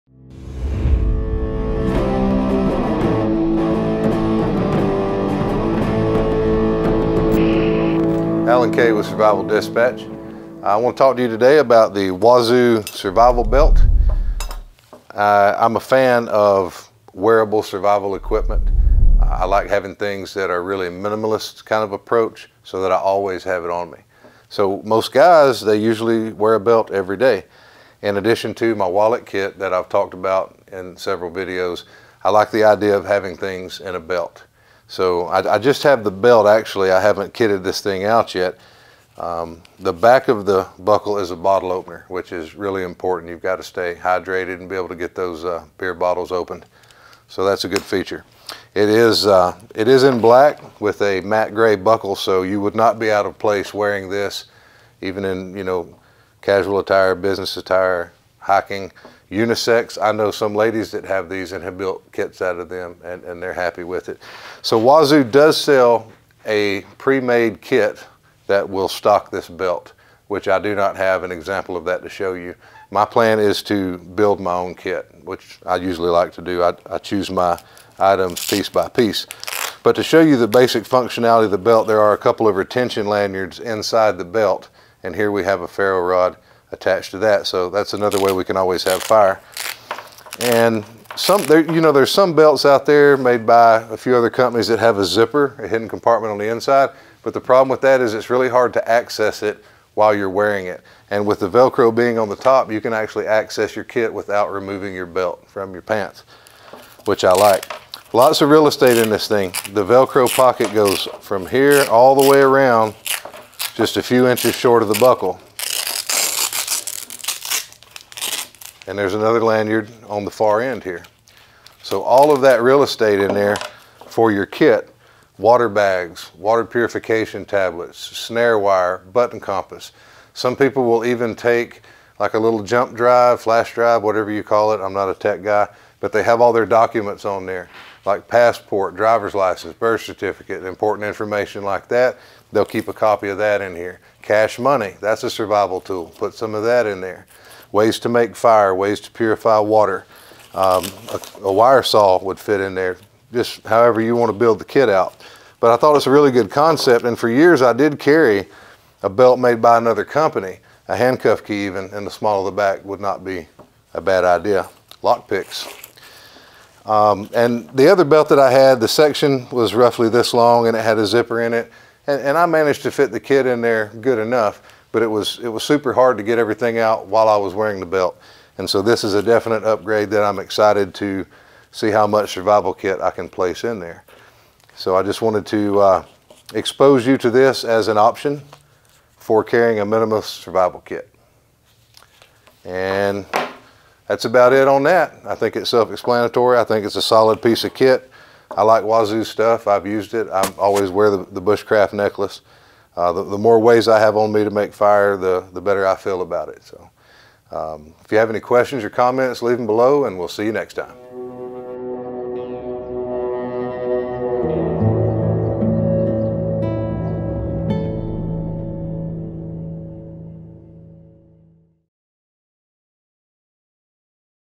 Wazoo Cache Belt
Having a few survival items on your person in case of an emergency is important. Most preppers have an EDC bag with some basic survival gear in it, and there are a variety of styles to choose from.

In this video, Alan Kay reviews the Wazoo Survival Belt which can be used not only as a belt for your pants, but also to store a few survival items like a fire kit, money, and ID in hidden compartments.